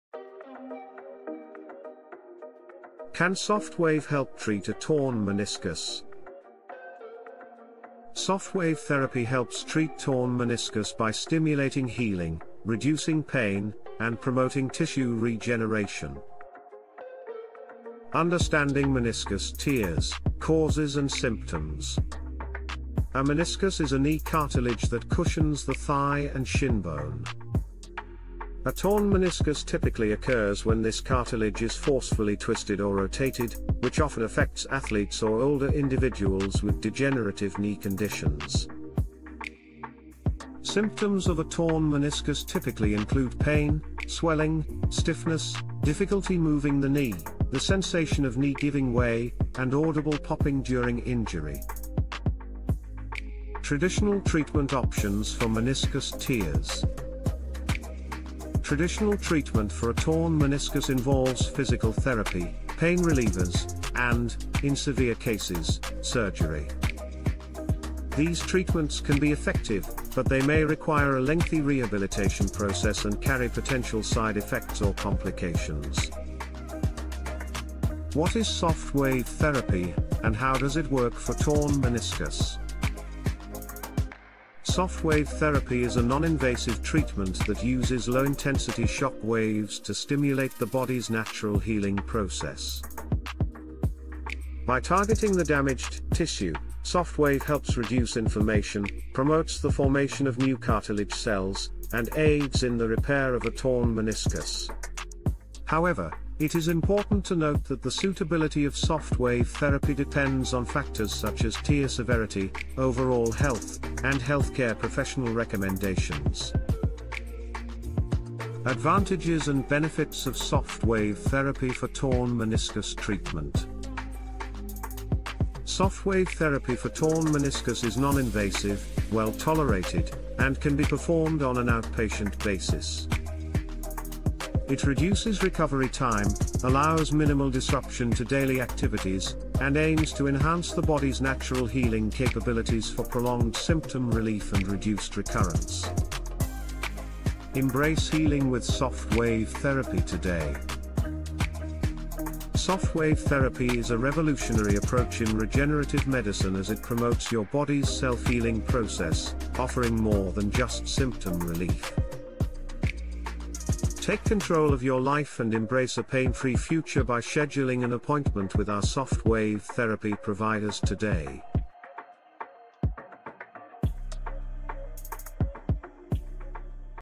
Can SoftWave Help Treat a Torn Meniscus?
Discover the potential of SoftWave Therapy as a treatment for torn meniscus. Learn how this innovative non-invasive approach can promote healing and provide relief from pain.